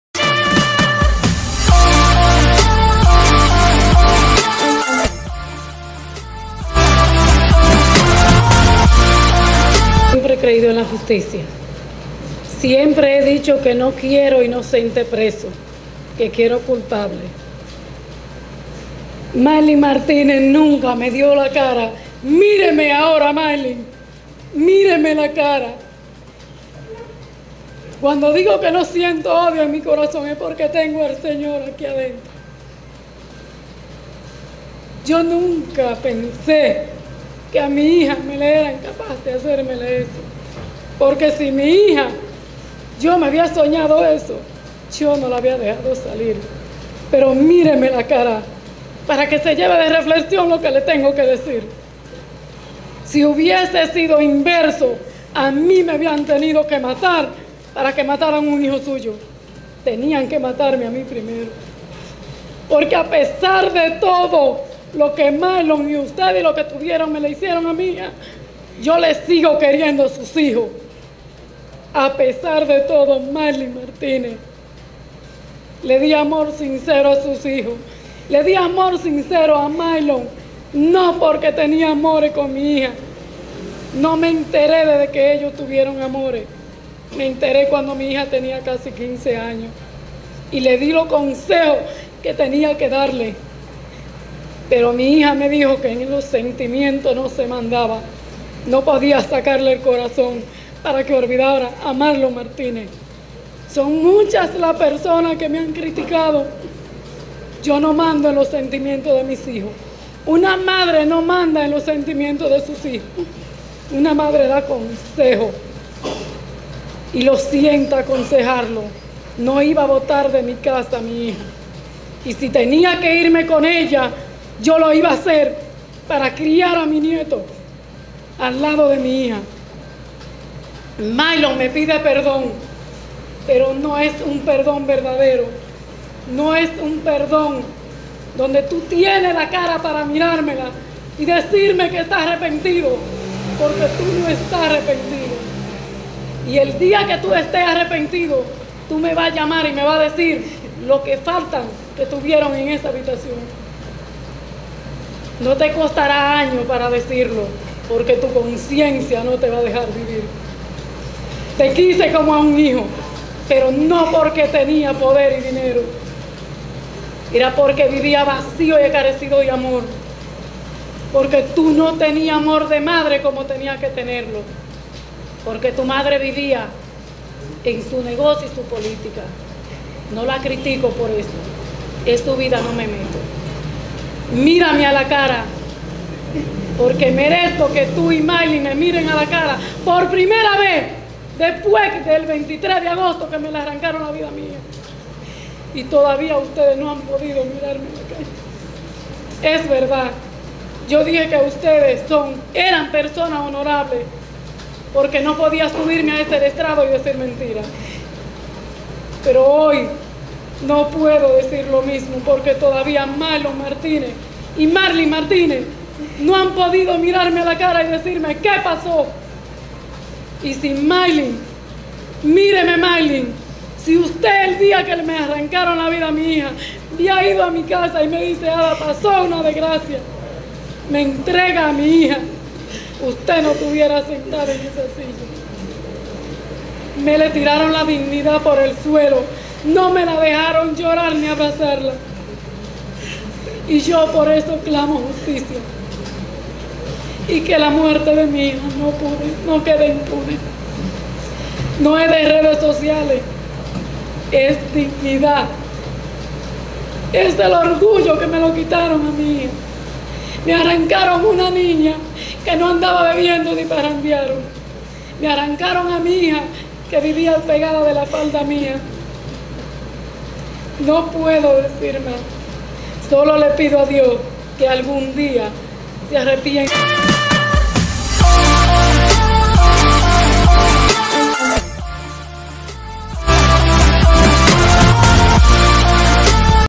😥La Madre de Emely Peguero Otorga 'que le mire la cara ' a Marlin y a Marlon MARTINEZ
Una  Pagina para los DOMINICANOS:

La Madre de Emely Peguero Otorga que le miren la cara a Marlin y a  Marlon MARTINEZ